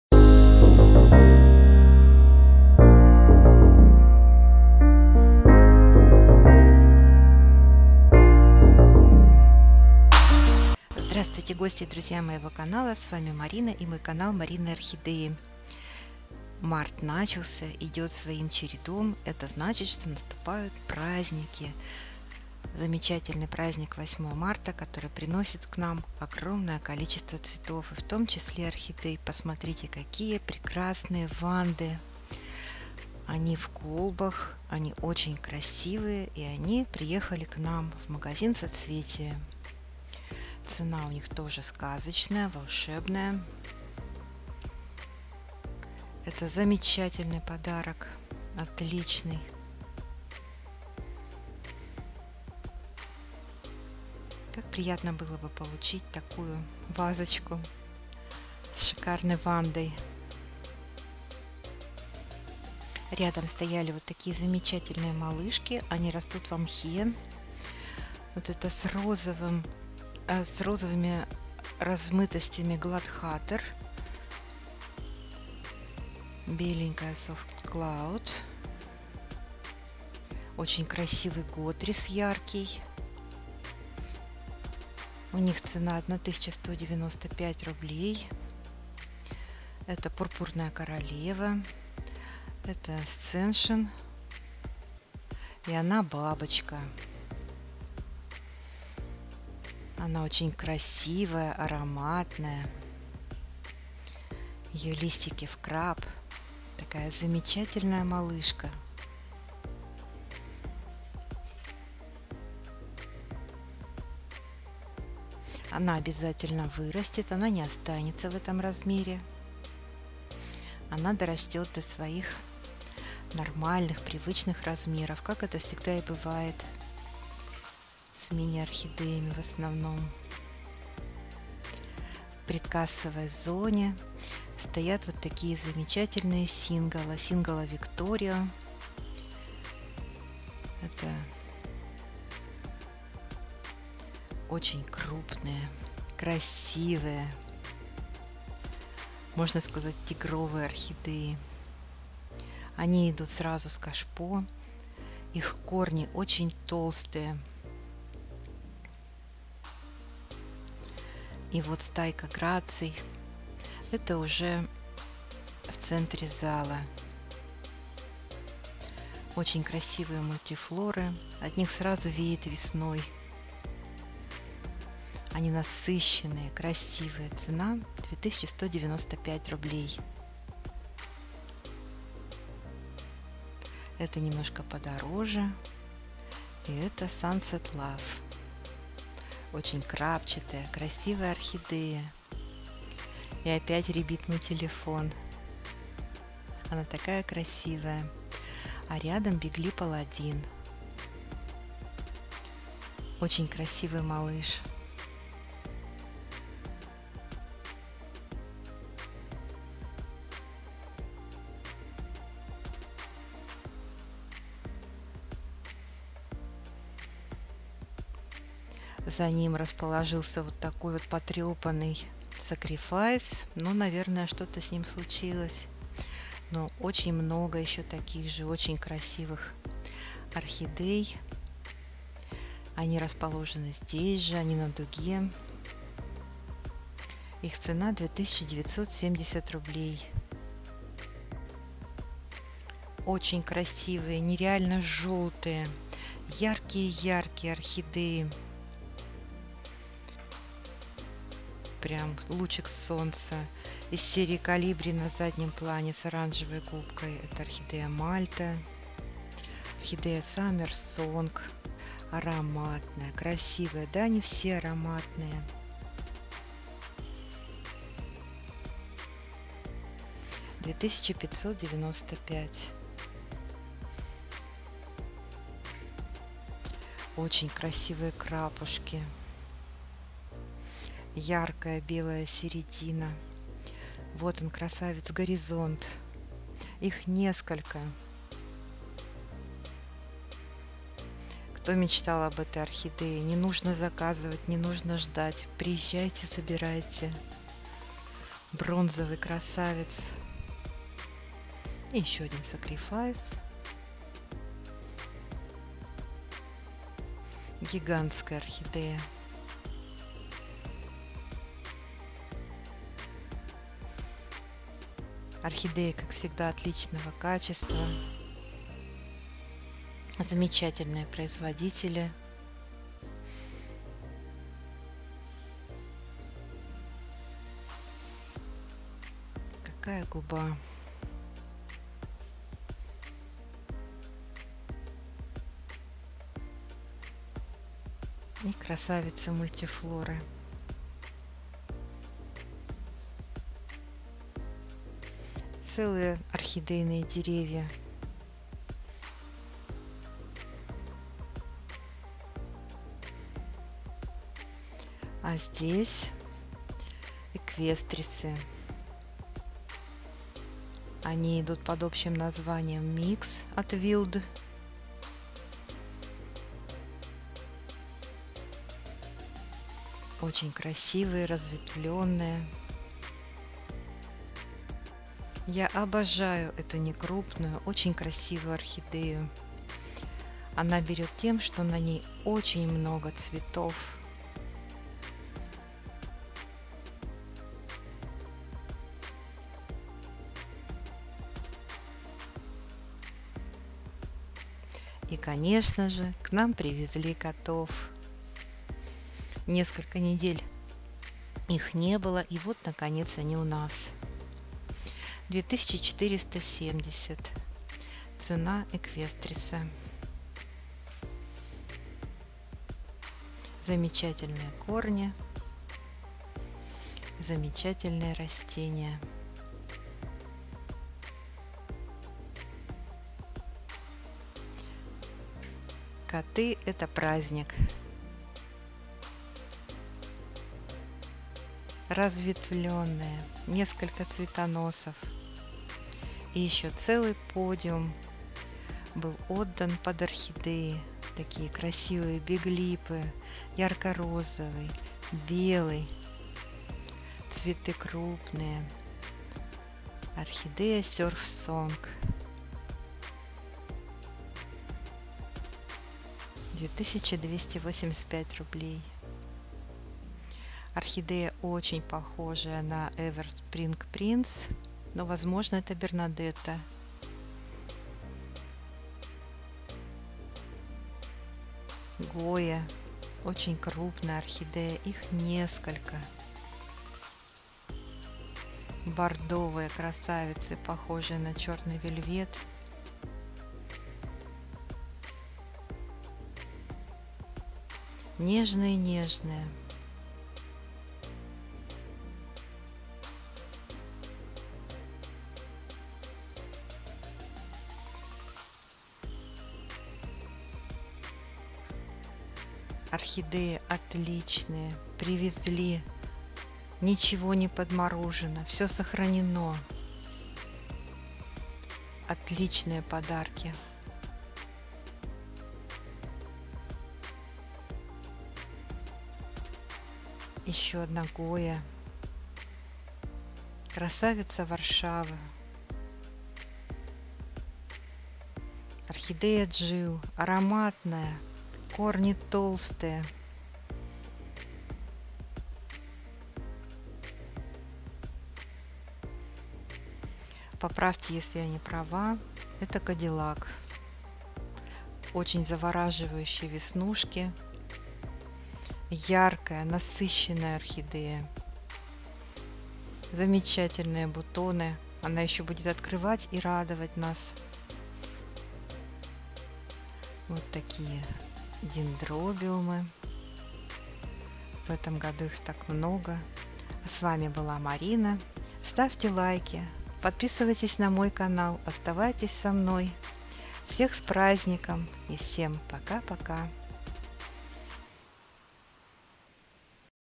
Огромный праздничный завоз орхидей в Соцветие Барнаул!
Море орхидей, которые очень хочется купить!